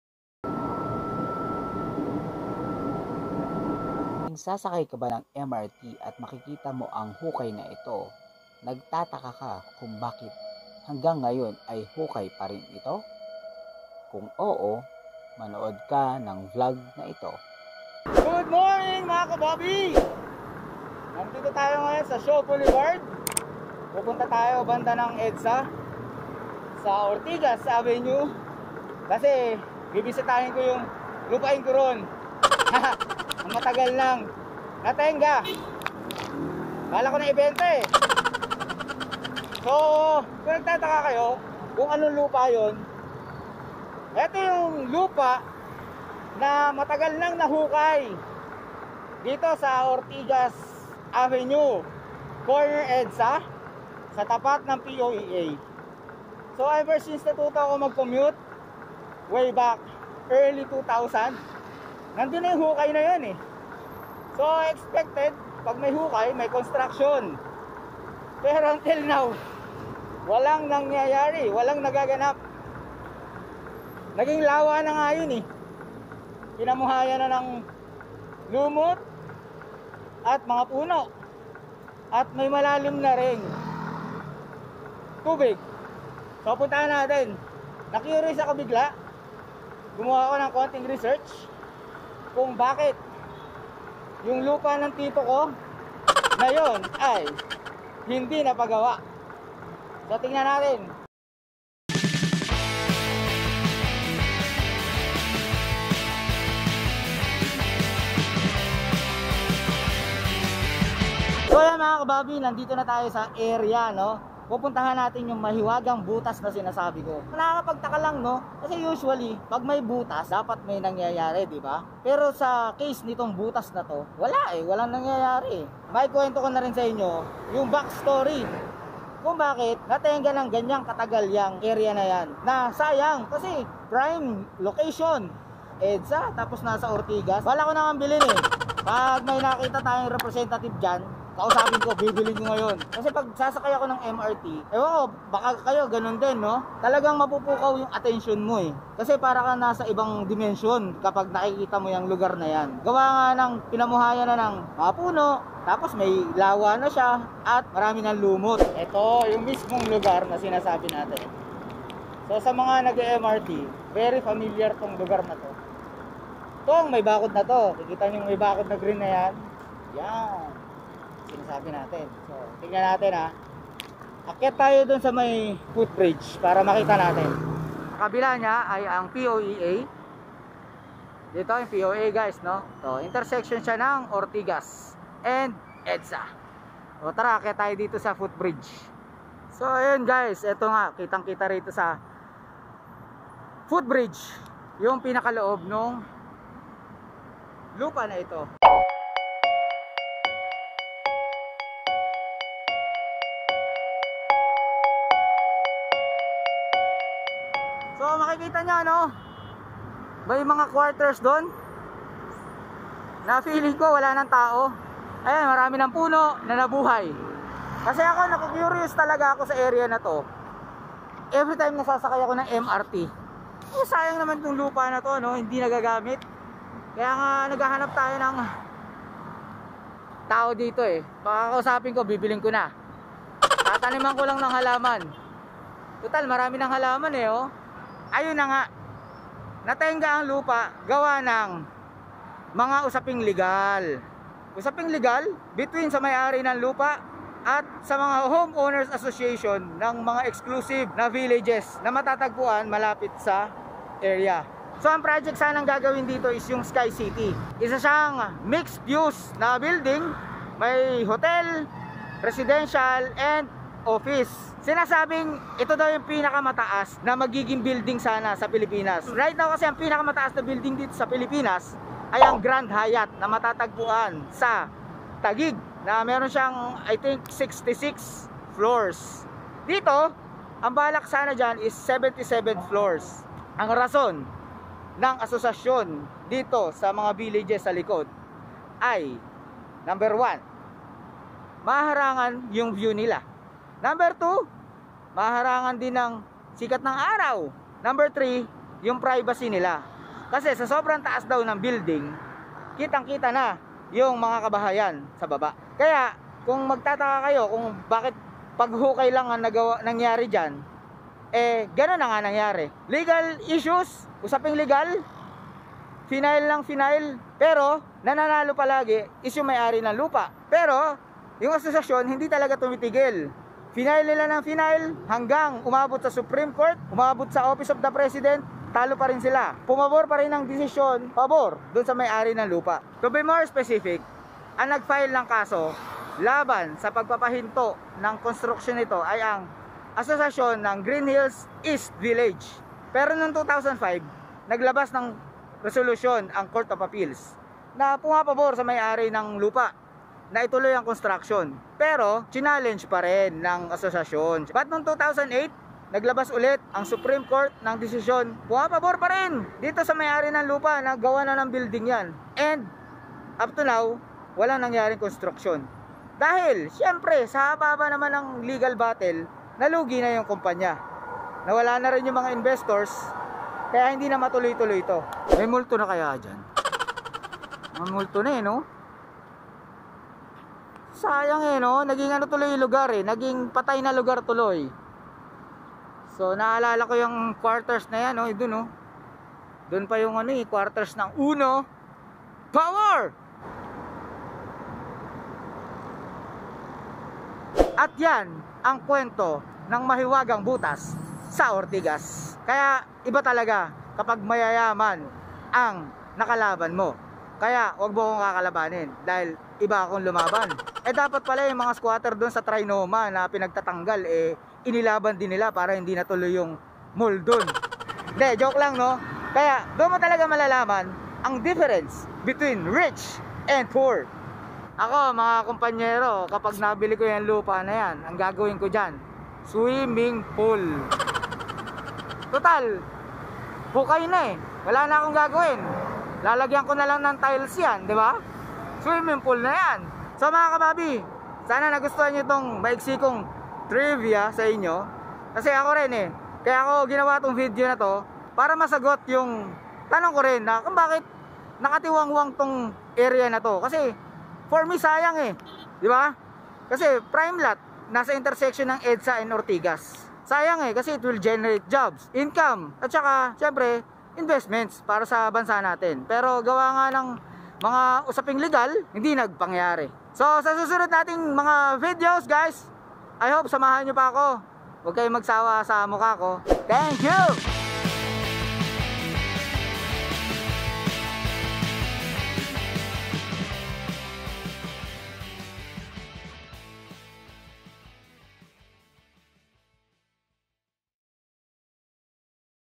Ang Mahiwagang Butas sa Ortigas
Isa ka rin ba sa mga nagtataka kung para saan at bakit nanatili na lamang na hukay ang lugar na ito? Kung oo ang iyong sagot watch mo na ang video na ito.